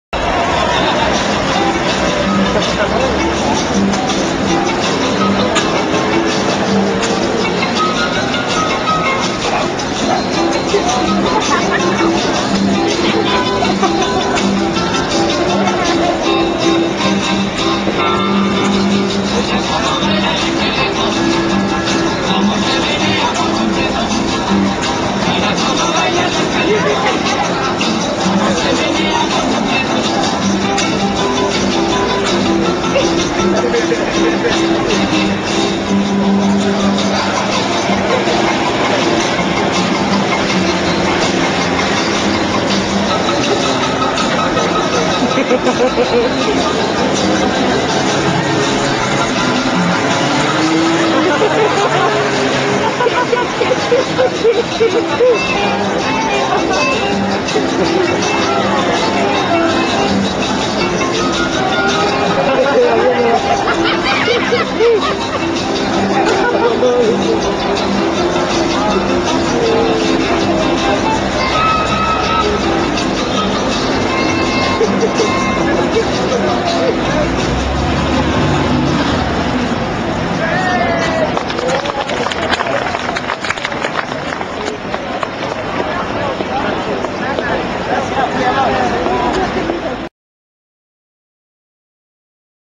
Marioneta Esqueleto
Curiosa y esplendida forma de pedir limosna con este esqueleto, todo un artista.